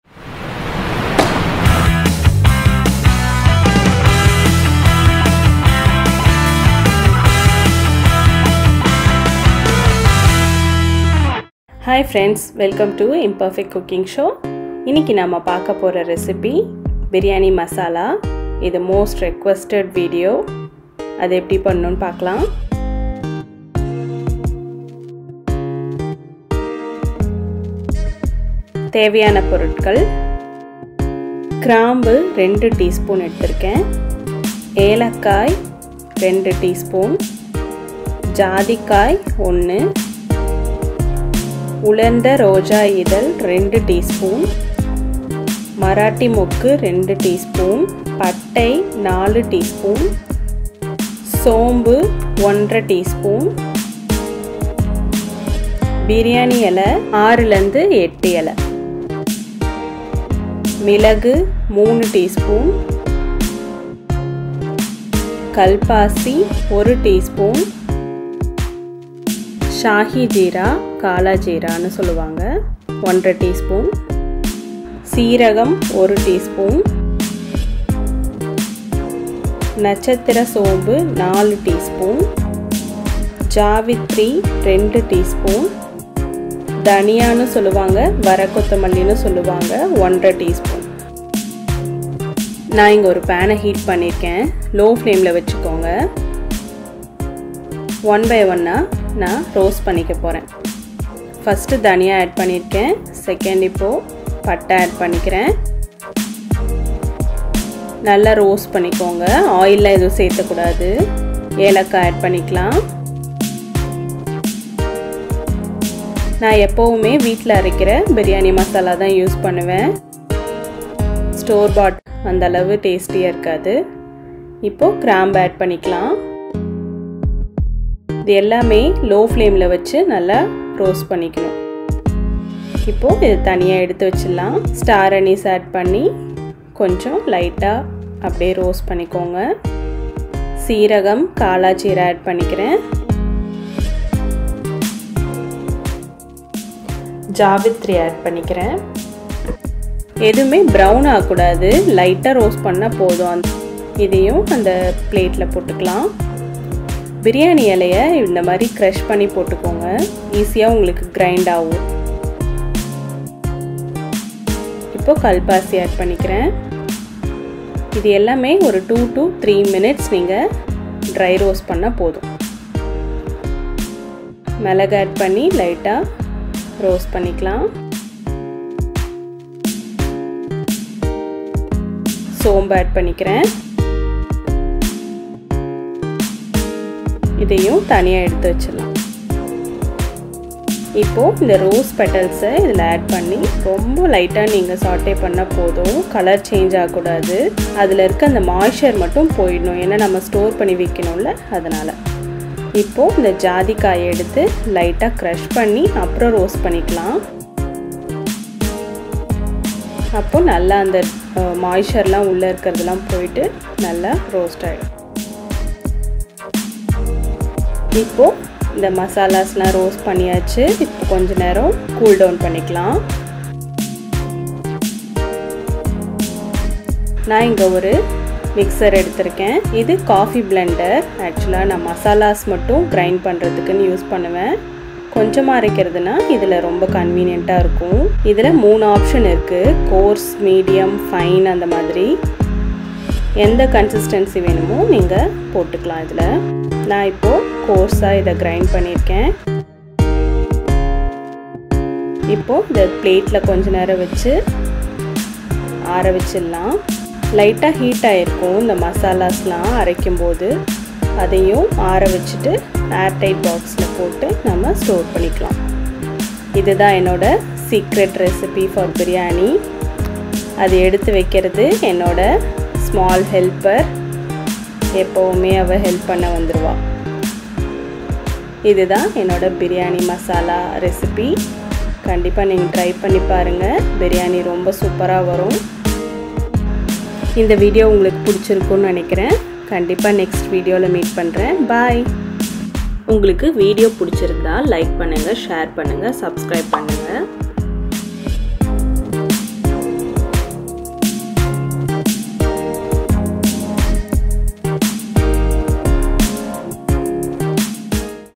BIRIYANI MASALA HOME MADE IN TAMIL / பிரியாணி மசாலா யின் தமிழ் / QUICK ,EASY AND TASTY
Biryani masala is a blend of whole spices lightly roasted and powdered. This is specifically used in biryani's as this masala powder adds not only flavour but immense aroma to the dish.